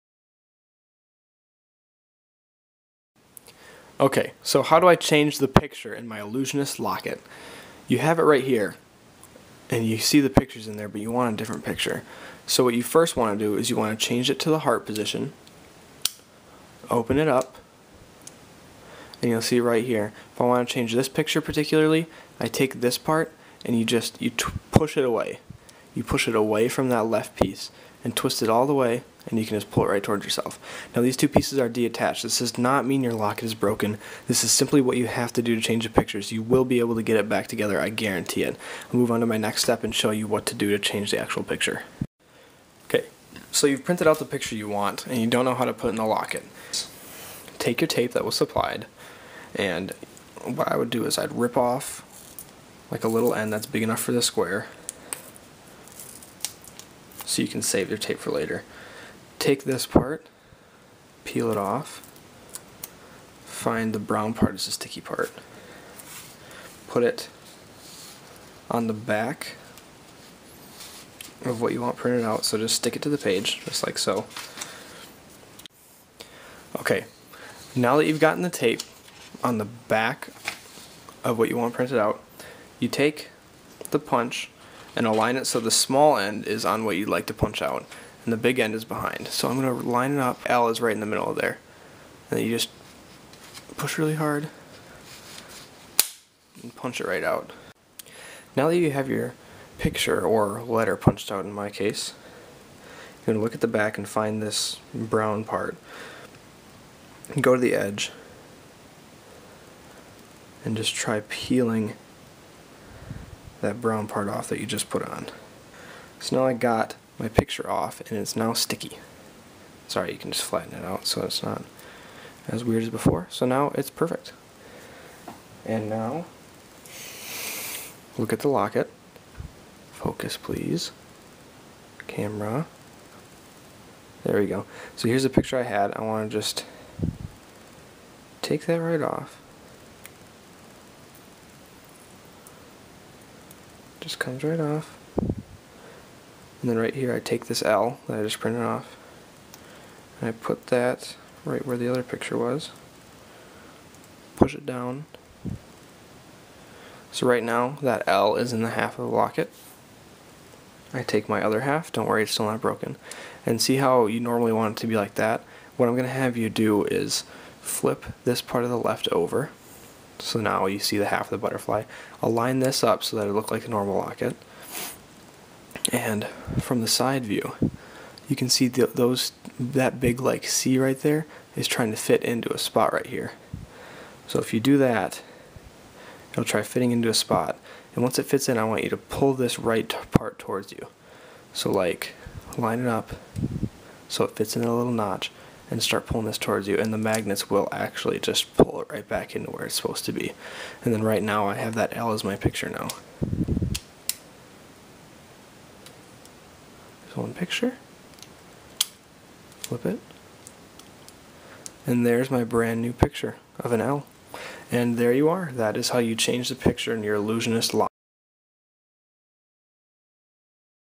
How To Change The Picture In Your Illusionist Locket Walkthrough
a quick how to change the picture in the illusionist locket from illusionlockets.com
it is a simple process that some people have a little trouble with it, so i am more than happy to show how it is done. you have to take the locket partially apart, but it is not broken. the magnets will always click it right into place. every time it will go back to where it started i guarantee it. if you have any questions about it please comment on my video your question before you contact illusionlockets. i can help you get your locket up and running in less than 5 minutes! please let me help you before worrying about your locket, it is not broken. i will be excited to help you all.